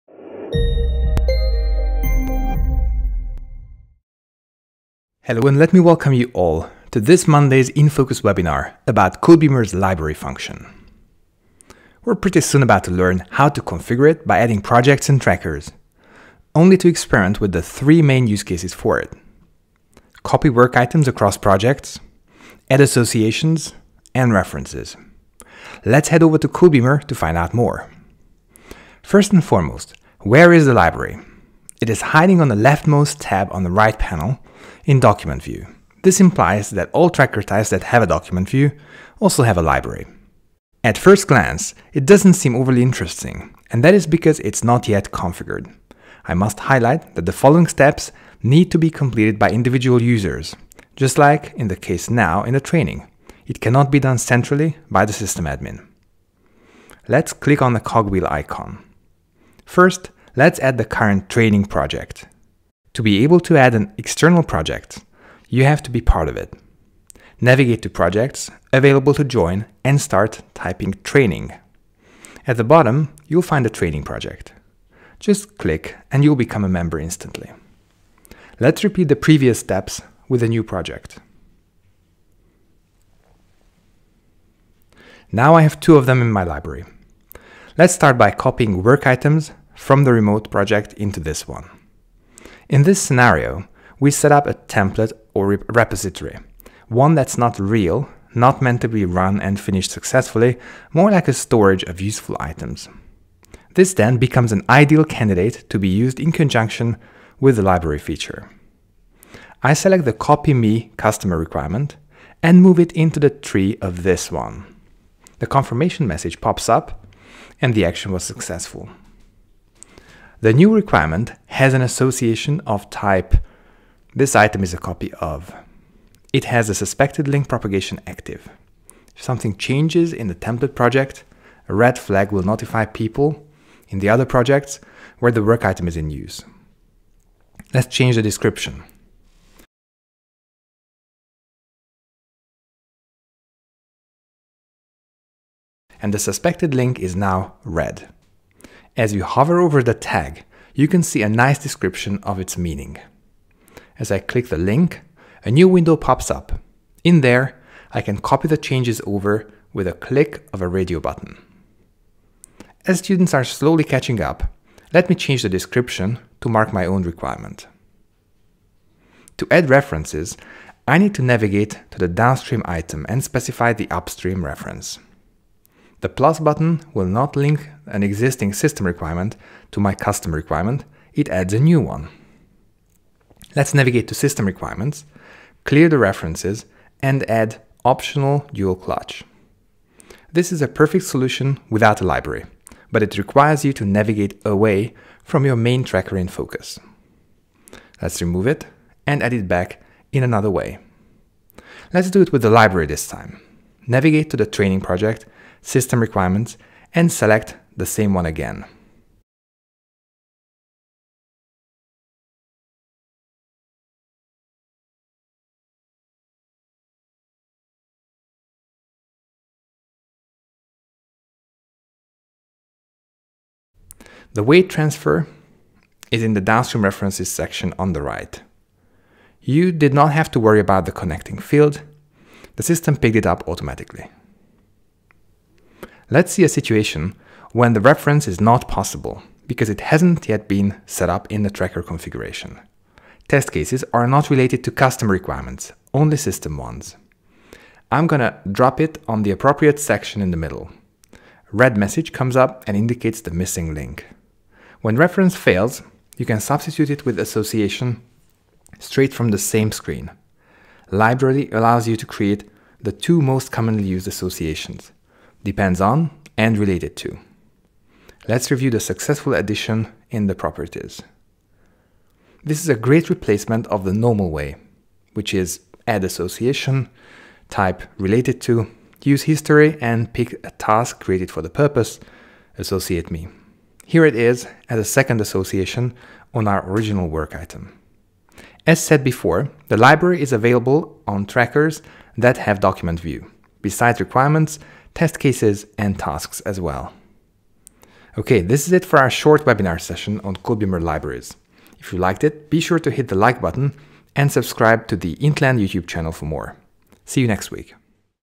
In Focus: The Library in codeBeamer ALM
In this video, you can learn how to configure the codeBeamer work item library, and how to use it to copy items, or to create associations and references.